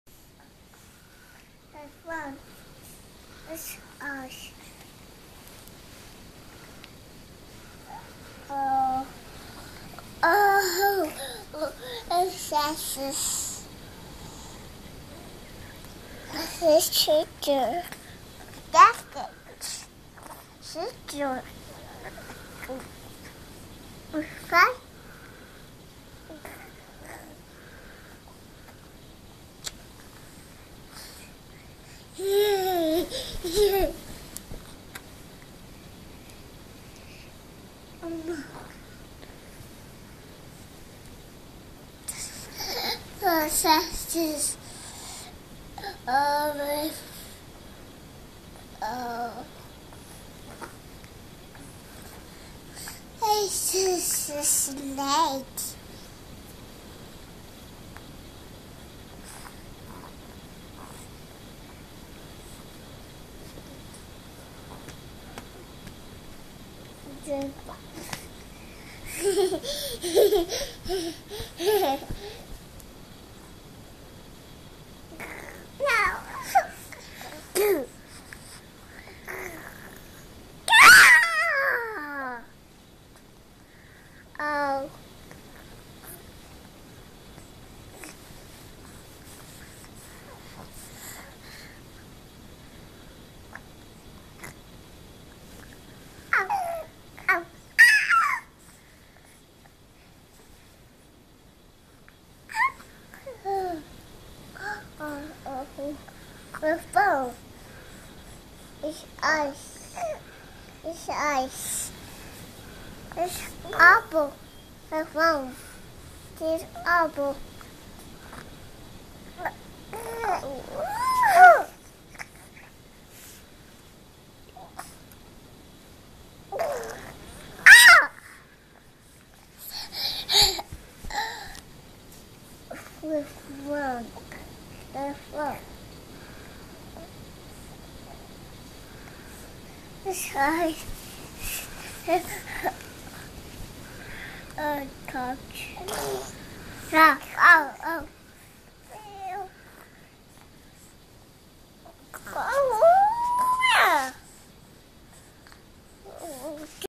Sister love and Baby talk
Audrey playing and talking with baby sister Alyssa.